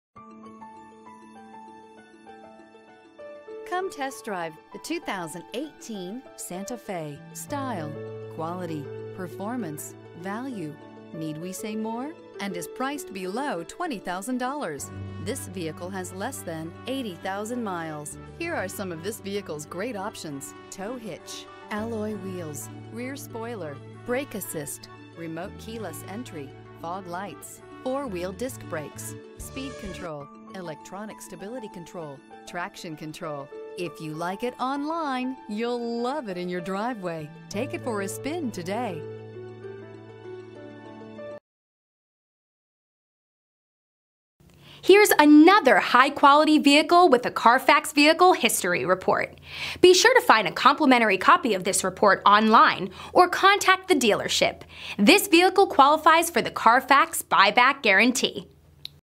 2018 Hyundai SANTA FE Sport Melbourne FL H85237A
2018 Hyundai SANTA FE Sport 2.4 Base

This 2018 Hyundai Santa Fe Sport 2.4 Base in Platinum Graphite features: FWD Clean CARFAX. Recent Arrival! Odometer is 1288 miles below market average! 21/27 City/Highway MPG Awards: * 2018 KBB.com 10 Most Awarded Brands * 2018 KBB.com 5-Year Cost to Own Awards Visit our virtual showroom 24/7 @